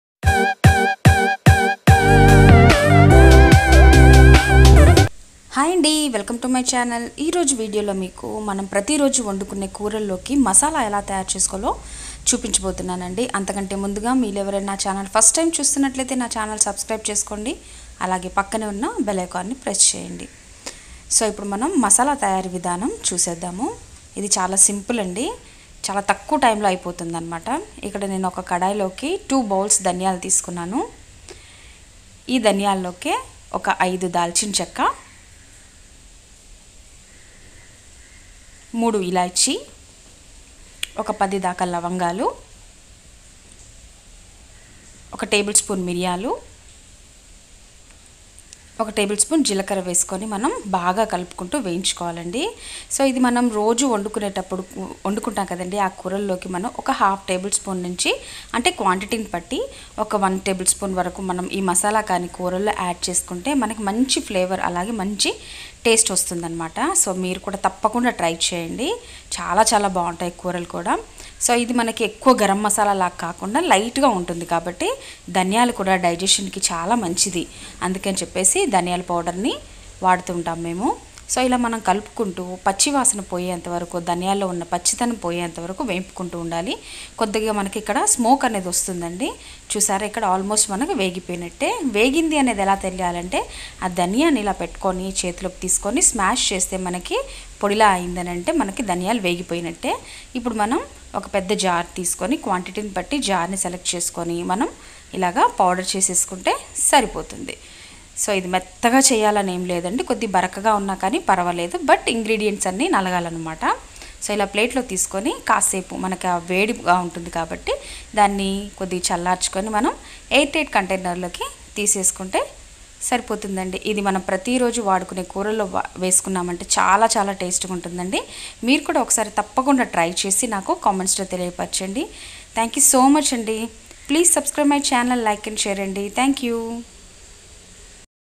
Curry powder|ఇలా చేసిన ధనియాల పొడి కూరల్లోకి వాడితే మంచి వాసన రుచి😋|Daniya powder recipe in telugu
Curry powder|ఇలా చేసిన ధనియాల పొడి కూరల్లోకి వాడితే మంచి వాసన రుచి😋|Daniya powder recipe in telugu by victoryvijaya

how to make daniya powder|curry powder recipe in telugu|daniya powder ela thayaru cheyali